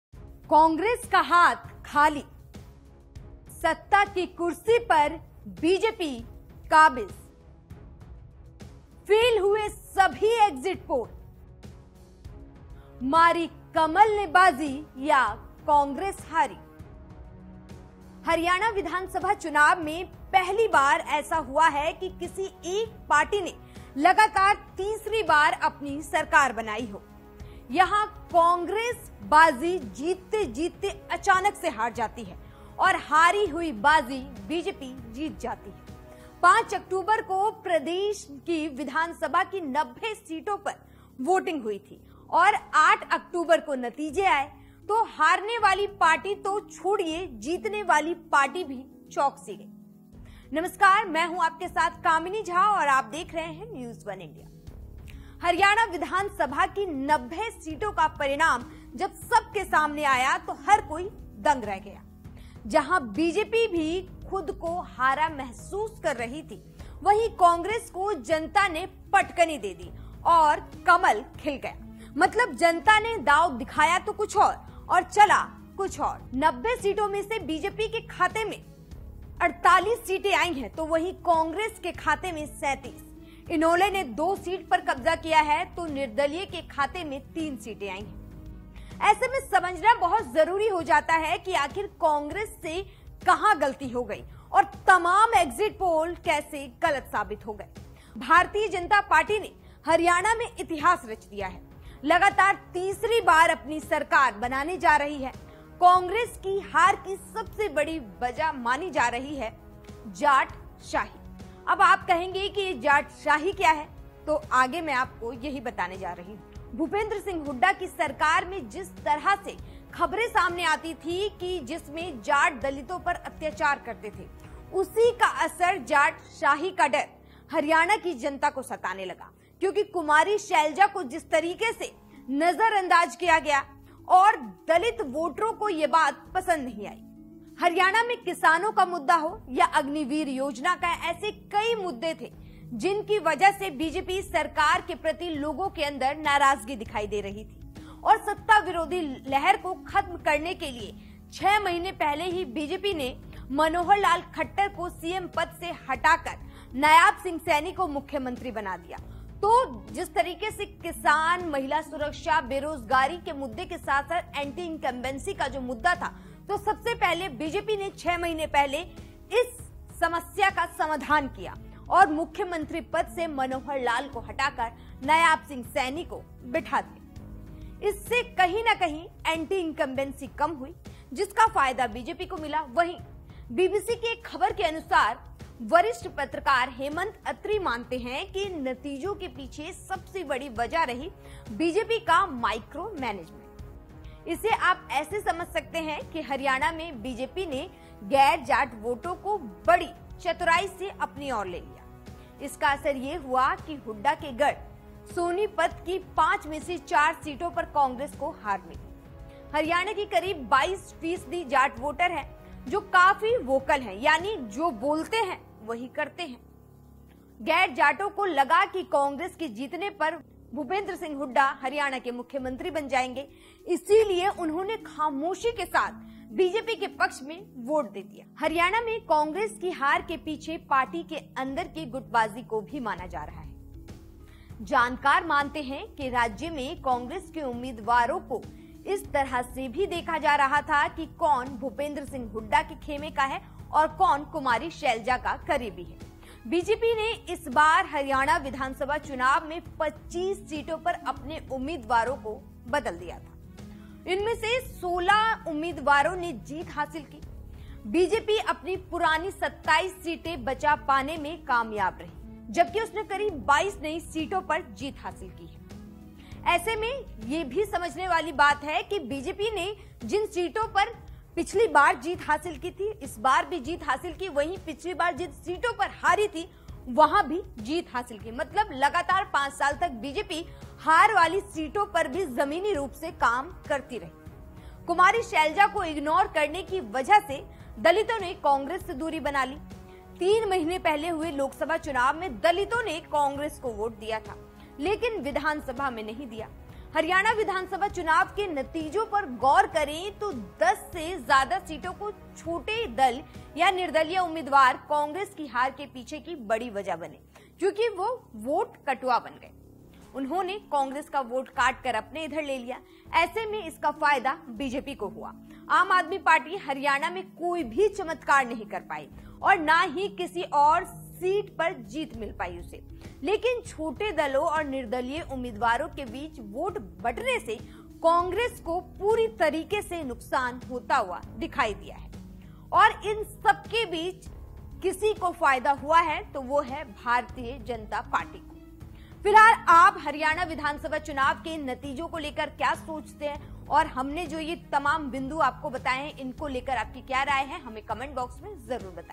Haryana Election Result 2024: हरियाणा में जाटशाही से डरी जनता कांग्रेस की हार के पीछे कौन !
Airtel - 350,
Netvision - 219,
RCN - 403,
UCN-223
Alisha Digital- 285
HDS- 127

We are also available on:
MX Player
JIO TV